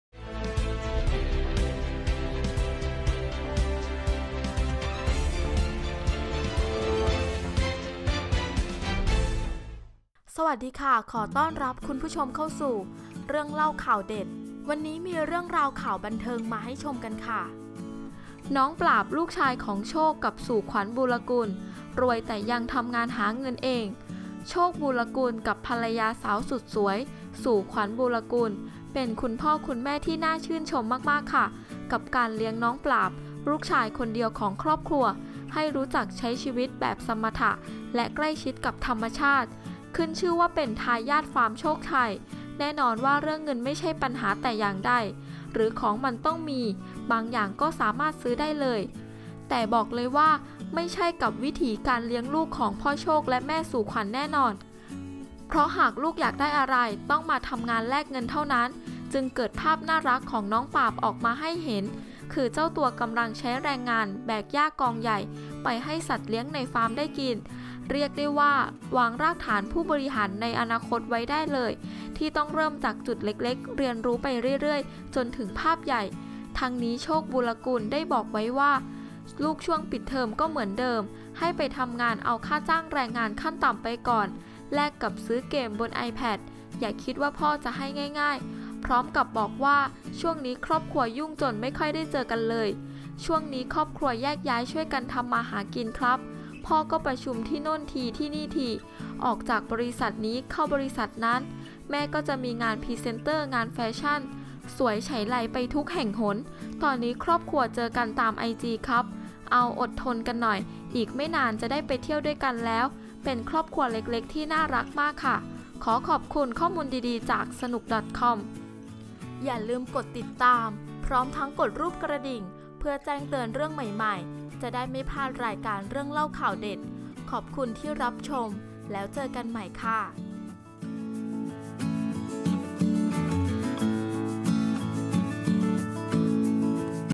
น้องปราบ ลูกชายของ โชค-สู่ขวัญ บูลกุล รวยแต่ยังทำงานหาเงินเอง
โชค บูลกุล กับภรรยาสาวสุดสวย สู่ขวัญ บูลกุล เป็นคุณพ่อคุณแม่ที่น่าชื่นชมมากๆ กับการเลี้ยงน้องปราบ ลูกชายคนเดียวของครอบครัว ให้รู้จักใช้ชีวิตแบบสมถะและใกล้ชิดกับธรรมชาติขึ้นชื่อว่าเป็นทายาทฟาร์มโชคชัย แน่นอนว่าเรื่องเงินไม่ใช่ปัญหาแต่อย่างใด หรือของมันต้องมีบางอย่างก็สามารถซื้อได้เลย แต่บอกเลยว่าไม่ใช่กับวิถีการเลี้ยงลูก ของพ่อโชคและแม่สู่ขวัญแน่นอน เพราะหากลูกอยากได้อะไร ก็ต้องมาทำงานแลกเงินเท่านั้น จึงภาพน่ารักของน้องปราบออกมาให้เห็น คือเจ้าตัวกำลังใช้แรงงานแบกหญ้ากองใหญ่ ไปให้สัตว์เลี้ยงในฟาร์มได้กิน เรียกว่าวางรากฐานผู้บริหารในอนาคตไว้ได้เลย ที่ต้องเริ่มจากจุดเล็กๆ เรียนรู้ไปเรื่อยๆ จนถึงภาพใหญ่ทั้งนี้ โชค บูลกุล ได้บอกไว้ว่า "ลูกช่วงปิดเทอมก็เหมือนเดิม ให้ไปทำงานเอาค่าจ้างแรงงานขั้นต่ำไปก่อน แลกกับซื้อ game บน ipad อย่าคิดว่าพ่อจะให้ง่ายๆ  พร้อมกับบอกว่า ช่วงนี้ครอบครัวยุ่งจนไม่ค่อยได้เจอกันเลย"ช่วงนี้ครอบครัวแยกย้ายช่วยกันทำมาหากินครับ พ่อก็ประชุมที่โน่นที ที่นี่ที ออกจากบริษัทนี้เข้าบริษัทนั้น แม่ก็จะมีงานพรีเซนเตอร์ งานแฟชั่น สวยไฉไลไปทุกแห่งหน ตอนนี้ครอบครัวเจอกันตาม IG ครับ
เอ้า อดทนหน่อย อีกไม่นานจะได้ไปเที่ยวด้วยกันแล้ว"
เป็นครอบครัวเล็กๆ ที่น่ารักมาก

ขอขอบคุณข้อมูลดีๆจาก : sanook.com